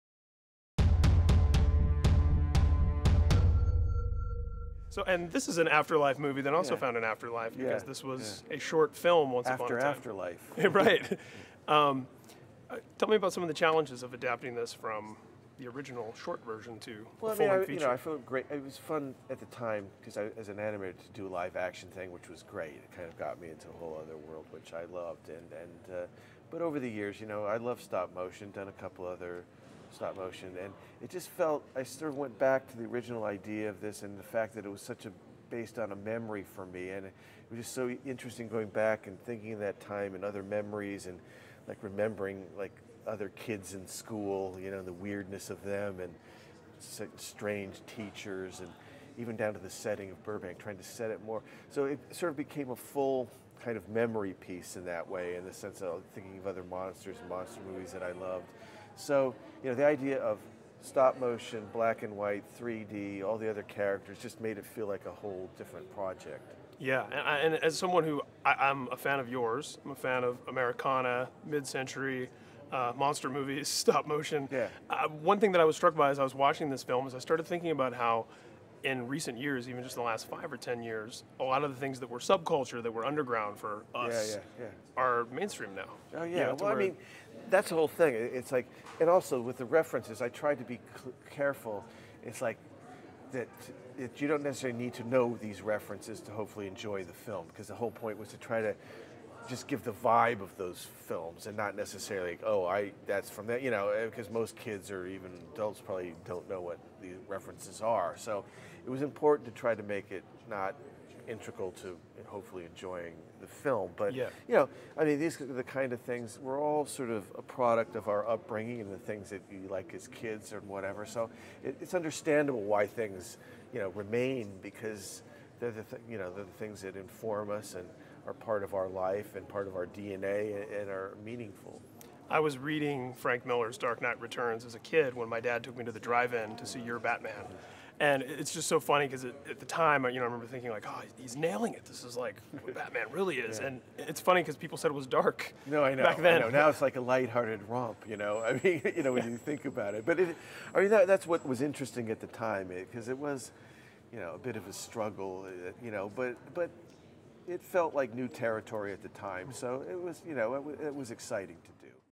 Frankenweenie Exclusive: Tim Burton
We go one-on-one with writer/director Tim Burton to talk about Frankenweenie.